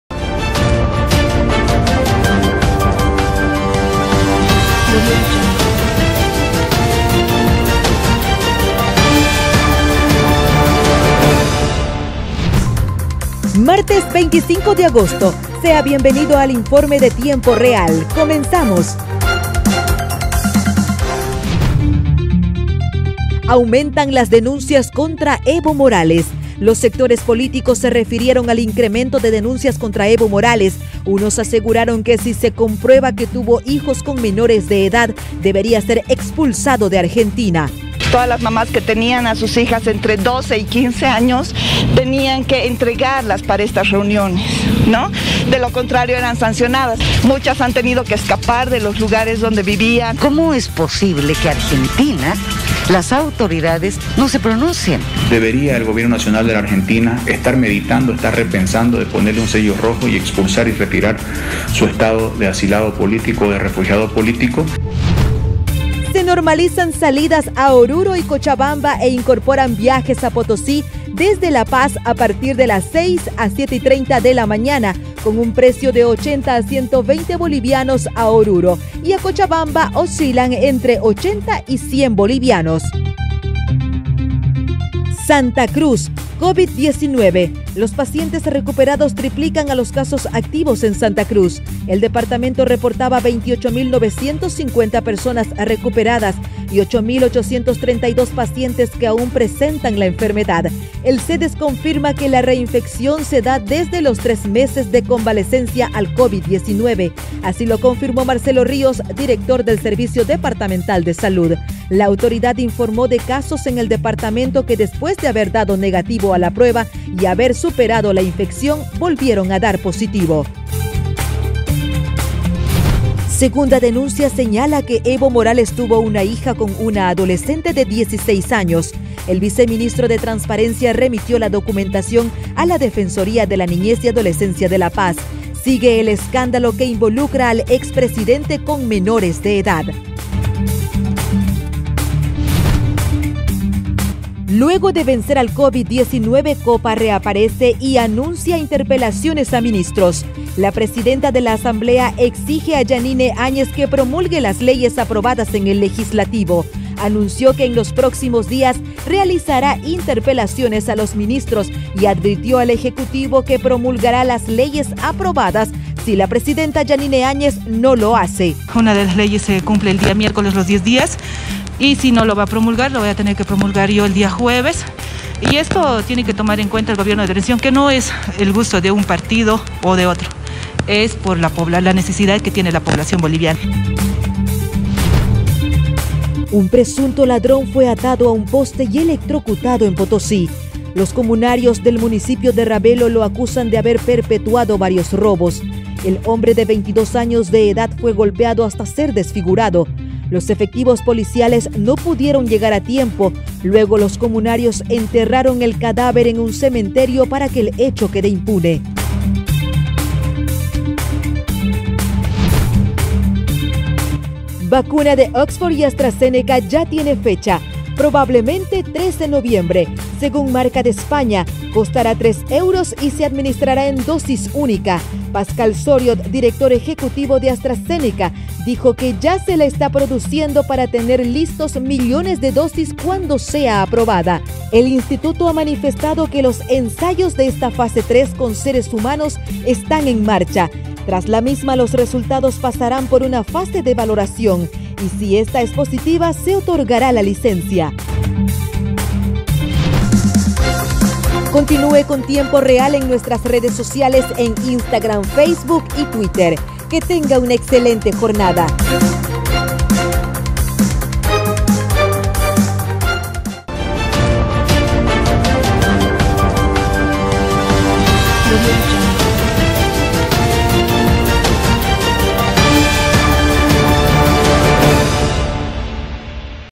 TIEMPO REAL NOTICIAS 25 DE AGOSTO
SE NORMALIZAN SALIDAS A ORURO Y COCHABAMBA E INCORPORAN VIAJES A POTOSÍ DESDE LA PAZ
AUMENTAN LAS DENUNCIAS CONTRA EVO MORALES
 LOS SECTORES POLÍTICOS SE REFIRIERON AL INCREMENTO DE DENUNCIAS CONTRA EVO MORALES,
SANTA CRUZ.-
COVID-19: LOS PACIENTES RECUPERADOS TRIPLICAN A LOS CASOS ACTIVOS EN SANTA CRUZ, EL DEPARTAMENTO REPORTABA 28.950 PERSONAS RECUPERADAS Y 8.832
SEGUNDA DENUNCIA SEÑALA QUE EVO TUVO UNA HIJA CON UNA ADOLESCENTE DE 16 AÑOS
LUEGO DE VENCER AL COVID-19, COPA REAPARECE Y ANUNCIA INTERPELACIONES A MINISTROS
POTOSÍ.-
UN PRESUNTO LADRÓN FUE ATADO A UN POSTE Y ELECTROCUTADO EN POTOSÍ
INTER.-VACUNA DE OXFORD Y ASTRA-ZENECA YA TIENE FECHA: PROBABLEMENTE 3 DE NOVIEMBRE